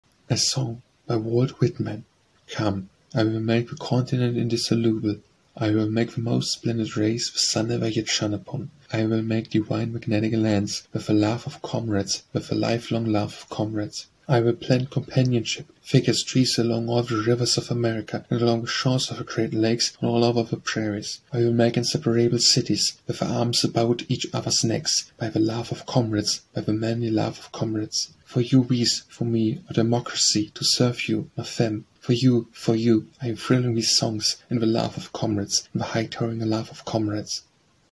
A Song - Walt Whitman
A Song

    By Walt Whitman

Come, I will make the continent indissoluble;
I will make the most splendid race the sun ever yet shone upon;
I will make divine magnetic lands,
With the love of comrades,
With the life-long love of comrades.

I will plant companionship thick as trees along all the rivers of
America, and along the shores of the great lakes, and all over
the prairies;
I will make inseparable cities, with their arms about each other's
necks;
By the love of comrades,
By the manly love of comrades.

For you these, from me, O Democracy, to serve you, ma femme!
For you! for you, I am trilling these songs,
In the love of comrades,
In the high-towering love of comrades.